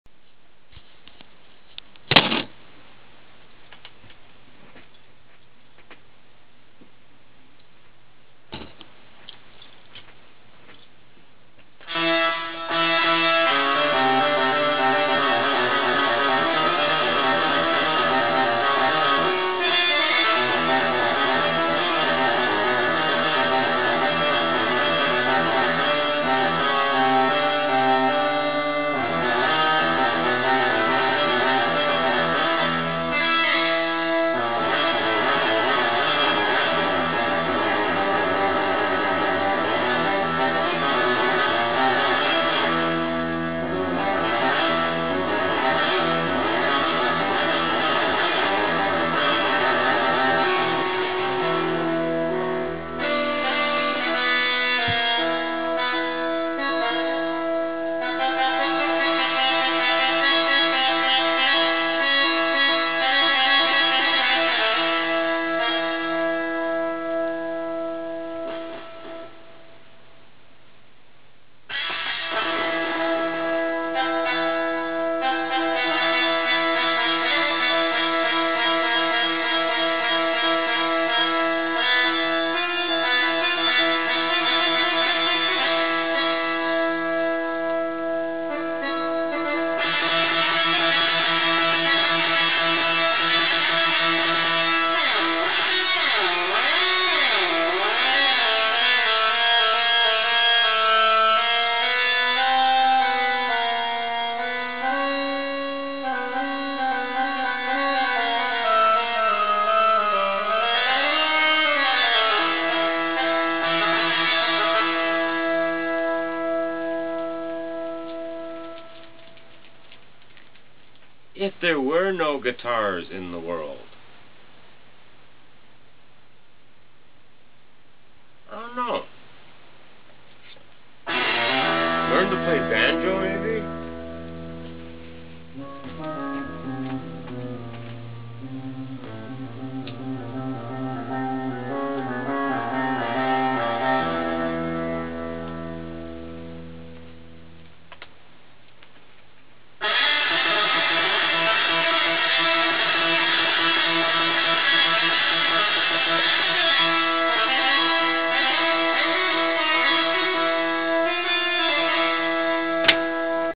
AcidJack =+=Paper Jamz medical use pt1
Acid Jack, Deacon Navitae vidfilm, produced, edited, directed by Pete Scottea. Music by Acid Jack ov the Acid-Jesus Phenomenon ov Brewer / Bangor (Brewbang) Maine. Note: best viewed by the psychedelikly enhanced. BotanicalSpirits.com recommended.